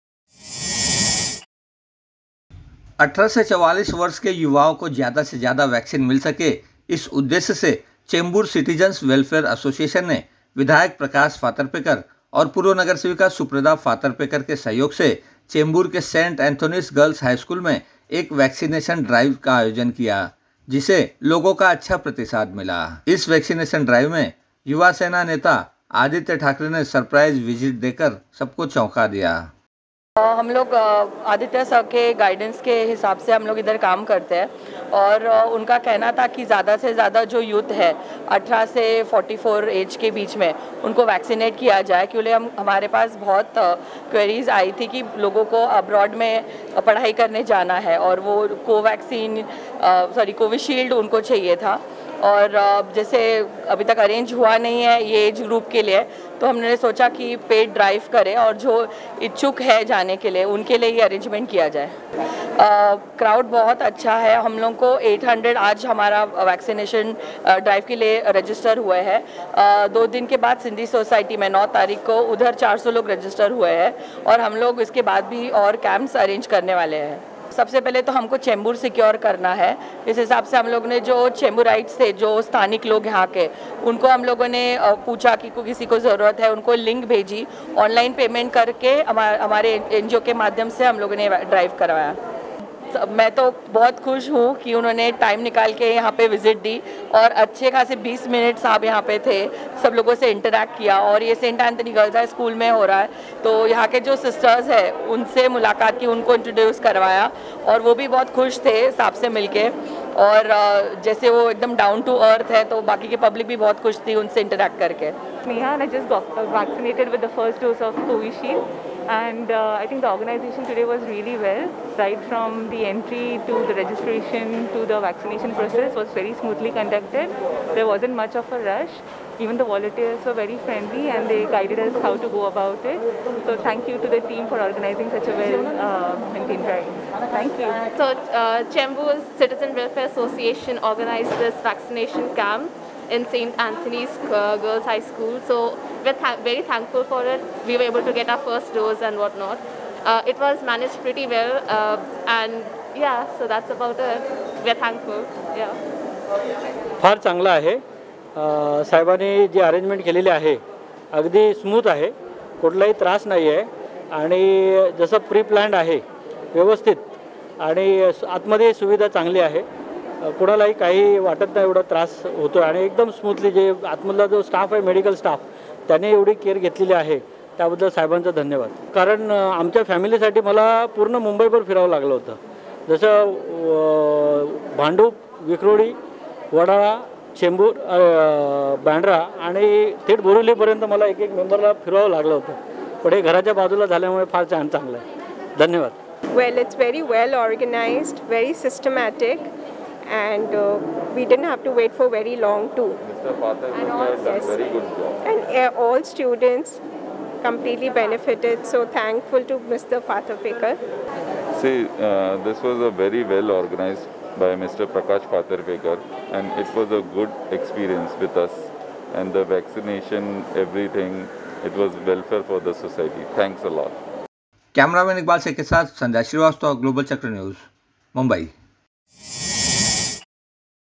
Ex Corporator Suprada Phaterpekar organised Vaccination Drive at Chembur gets huge response
विधायक प्रकाश फातरपेकर, पूर्व नगरसेविका सुप्रदा फातरपेकर और चेंबूर सिटिज़न्स वेलफेयर एसोसिएशन द्वारा चेंबूर के सेंट एंथोनी स्कूल में आयोजित वैक्सीनेशन ड्राइव को मिला अच्छा प्रतिसाद

MLA Prakash Phaterpekar, Ex Corporator Suprada Phaterpekar organised and Chembur Citizens Welfare Association Vaccination Drive at Chembur gets huge response